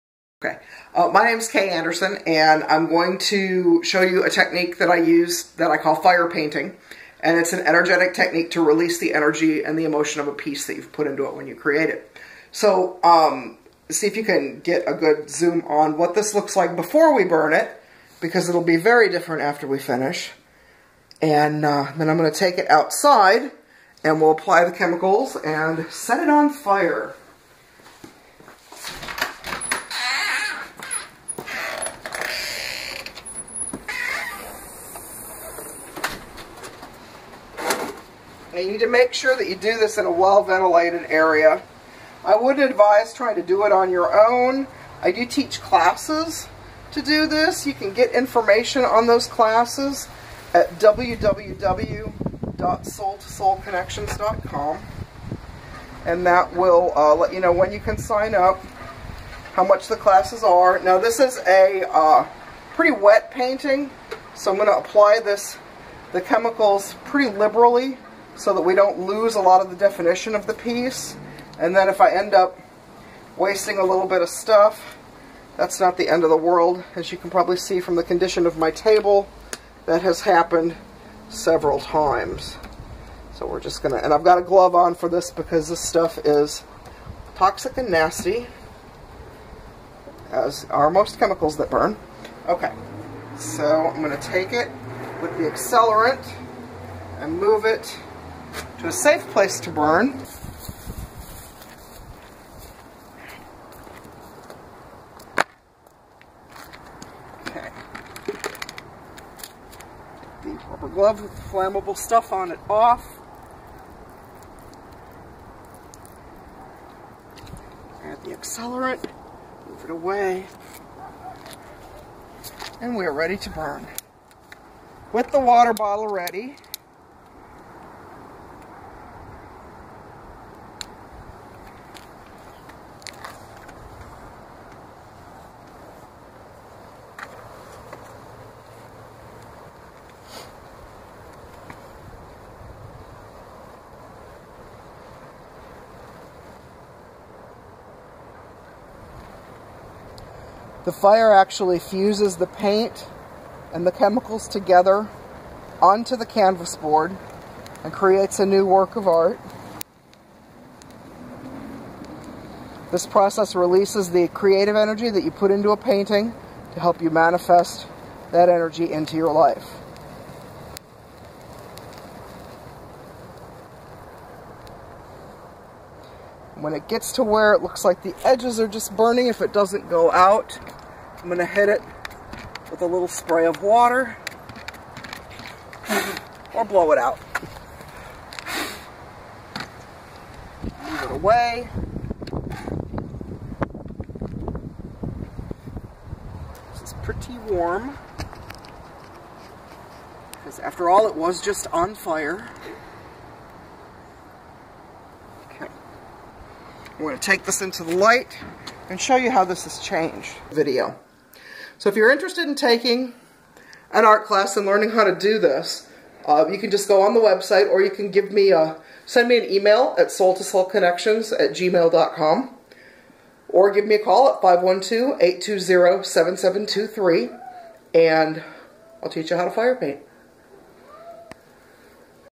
Elemental Alchemy (Fire Painting)
This is a process used to release the creative energy an artist pours into a piece of art.  It's an effective tool to release blocks to creativity.  The artist paints an abstract representation of the block and then literally sets it on fire, transforming the artwork.  Class descriptions and dates are available